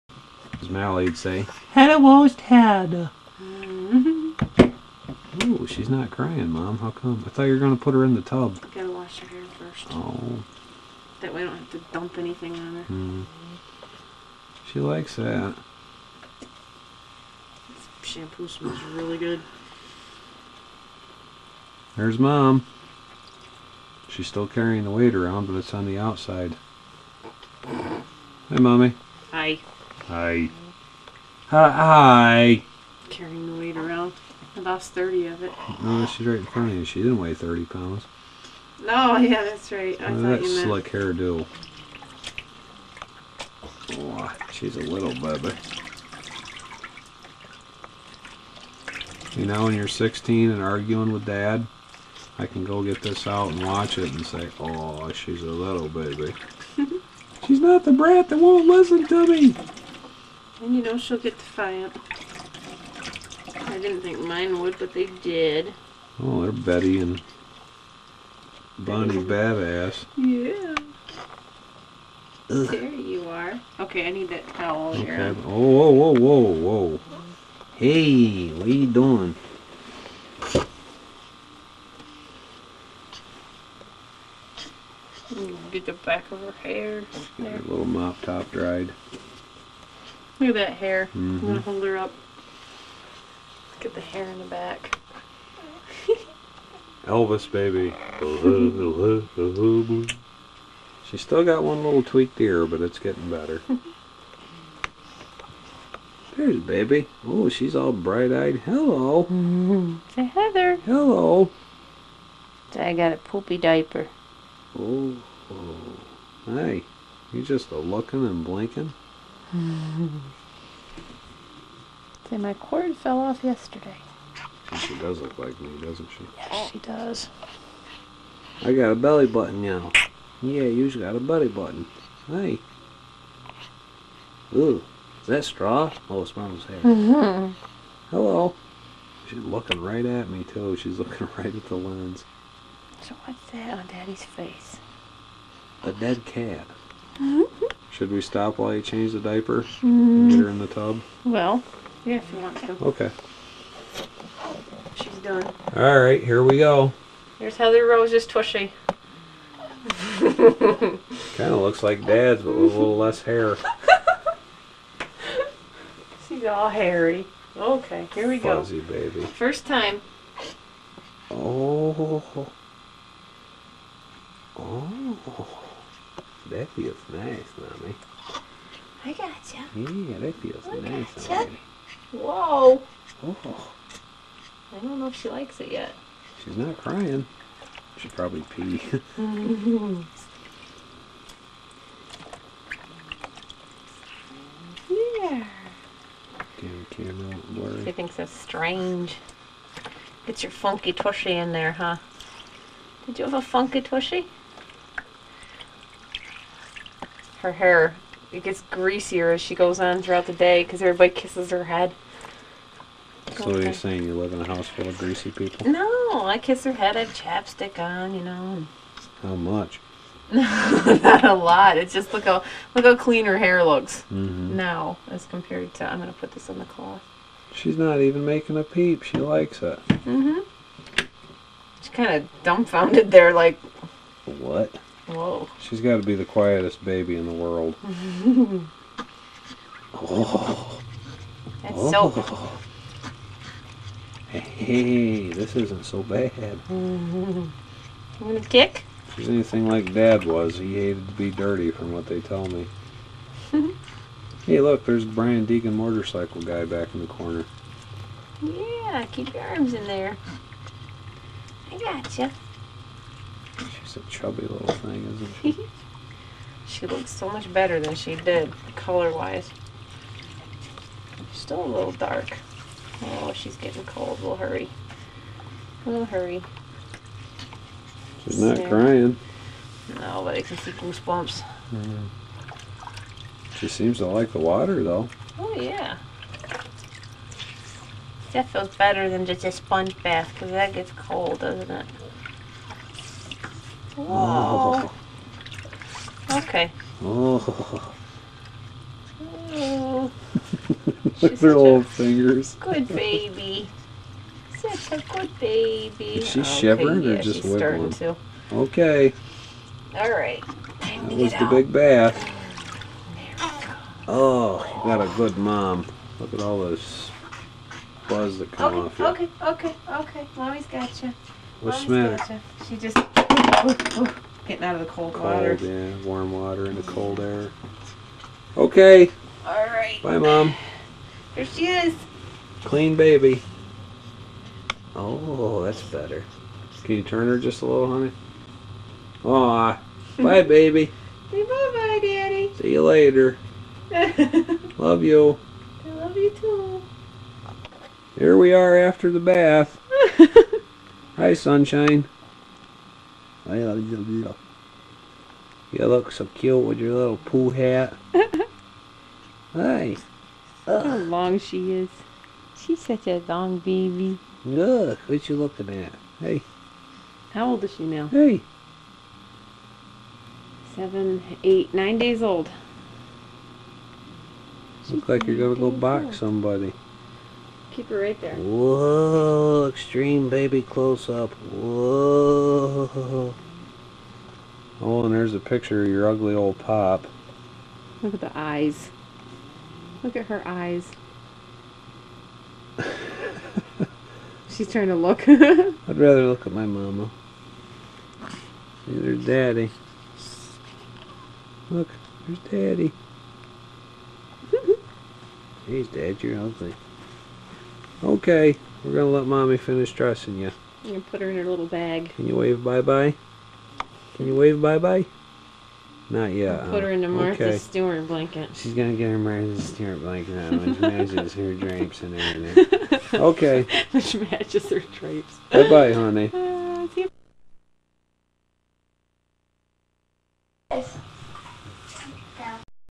Heather 1st Bath
Heather Roses's first baby bath with Daddy looking on. :)